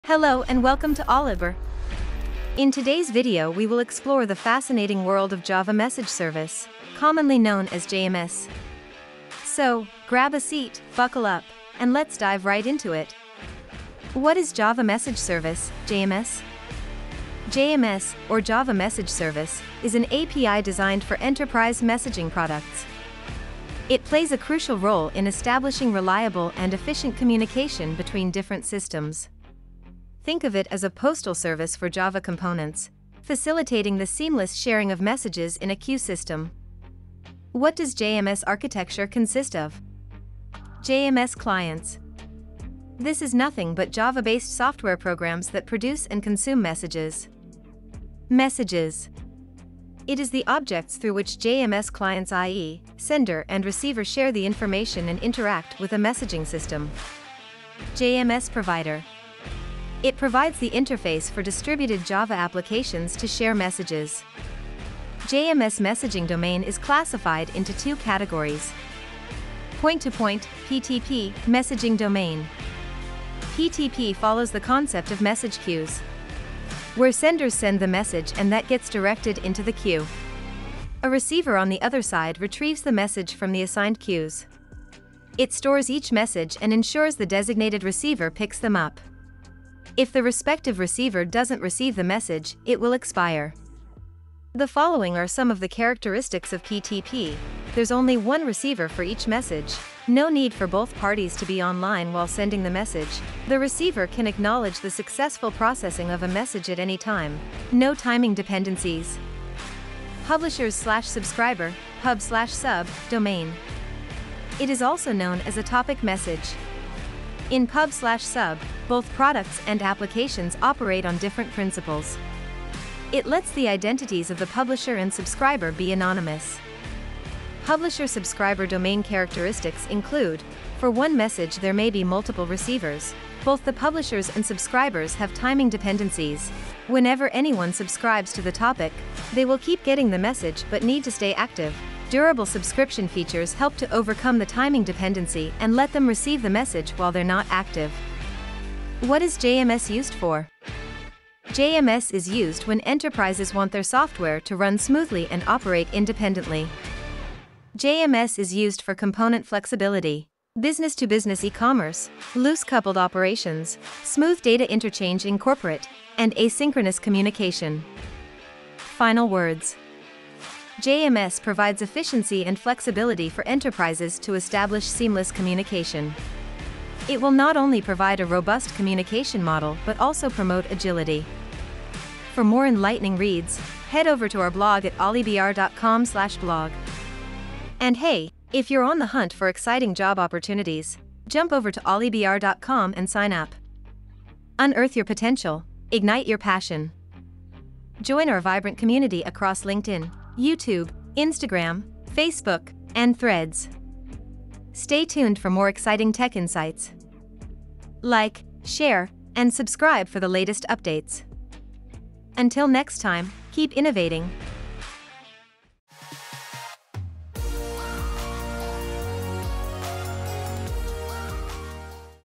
What is JMS? What is it used for?
JMS is a Java Message Service API for enterprise messaging products. It establishes communication between two different systems in a reliable and efficient way. In simpler words, JMS is like a “Postal Service” for Java components that allows the sharing of messages from one component to another component in a queue system. In this video, we will explore What is JMS? And What is It Used for? Let’s dive right into it!